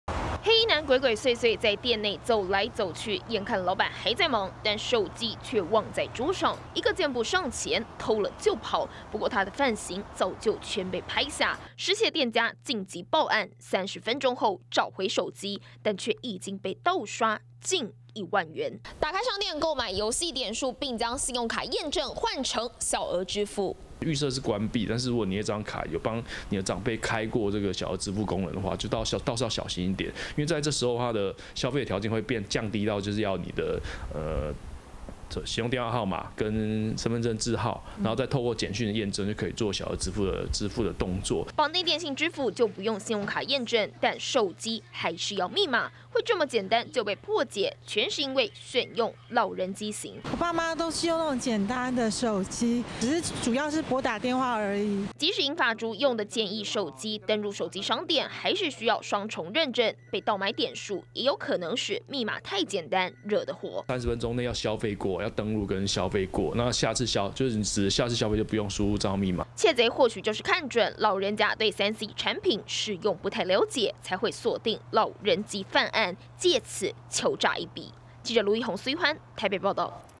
20190121中天新聞 鎖定銀髮族手機 盜刷上萬「遊戲點數」
桃園龜山一名攤商，在忙招呼客人的時候，竟然有一名男子偷偷將老闆手機偷走，半小時後在附近找到，卻已經被偷盜買遊戲點數一萬點，相當台幣一萬元，專家指出，許多長輩不太會用智慧型手機，因此都買簡易型，指紋或臉部辨識的功能都沒有，反而讓竊賊有機可趁。